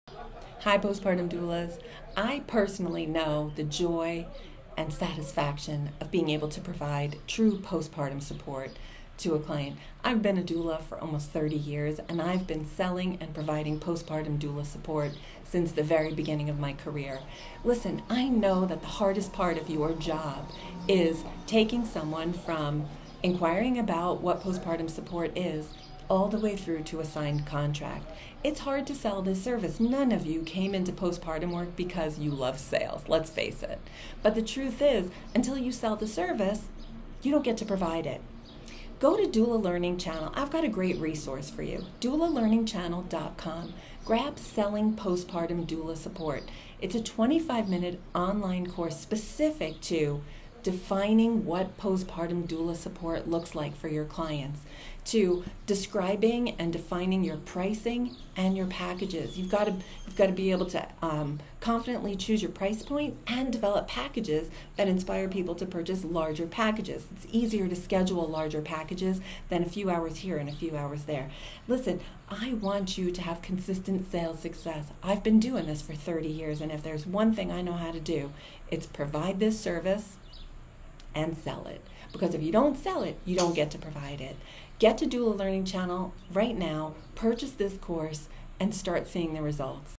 Selling Postpartum Doula Support - Doula Learning Channel
At the conclusion of this online learning module, learners will have the tools and skills to confidently describe their postpartum doula services, define their pricing, and develop their postpartum packages. Learners will have the ability to apply these skills during postpartum support selling opportunities to enable consistent sales success!

Follow @doulalearningchannel and @prodoula on Instagram for updates on new courses!